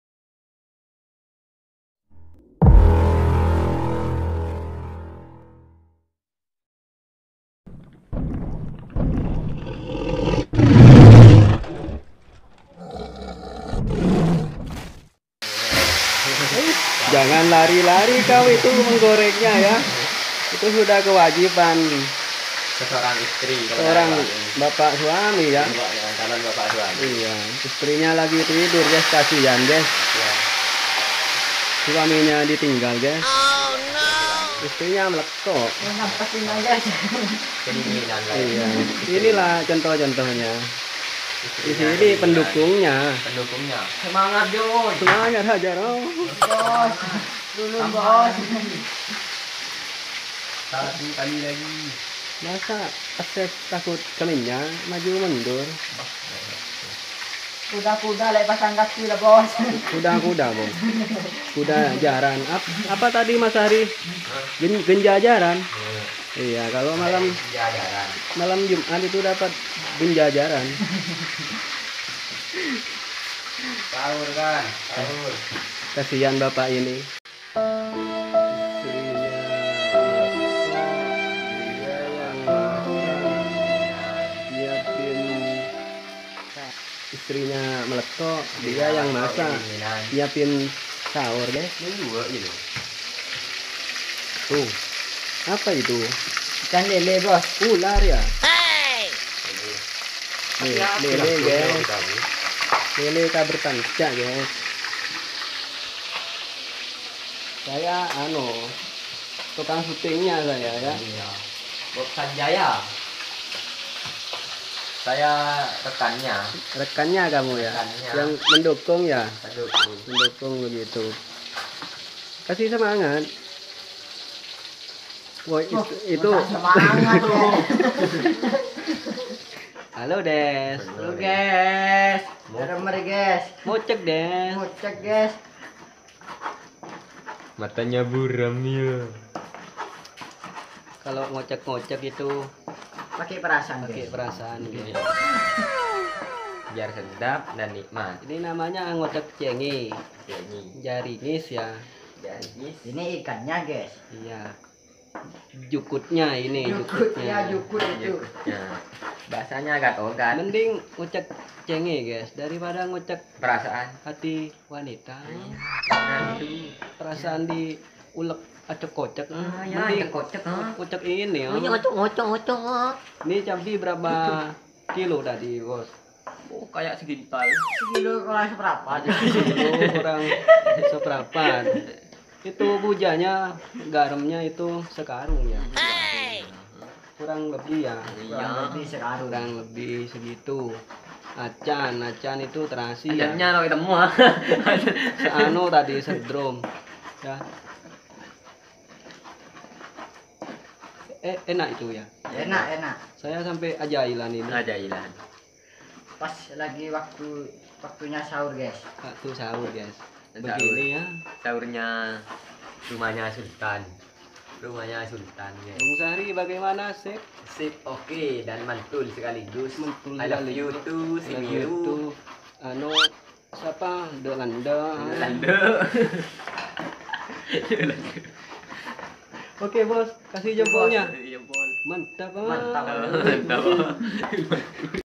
VLOG Ep. 27 MASAK PEDAS IKAN TANGKAPAN MANCING TADI SIANG
hidup di desa mancing dan masak ikan persiapan makan sahur dari hasil tangkapan...
Halo TEMAN-TEMAN
Bangai mana kabarnya? Pasti baik ya.
Trimakasih sudah mengunjungi cahnnel kami